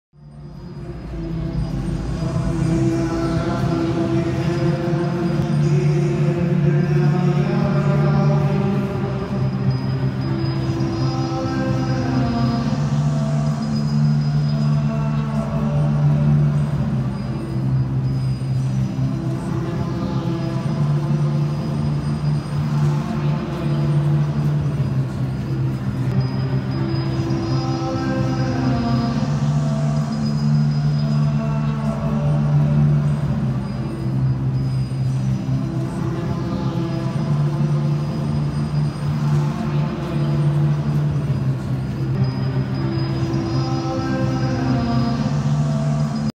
Durham Lumiere 2011 & 2019 Spirit by Compagnie Carabosse
Countless cotton vests are lit with candles and suspended from the ceiling inside Durham Cathedral. An installation part of Durham Lumiere 2011 & 2019.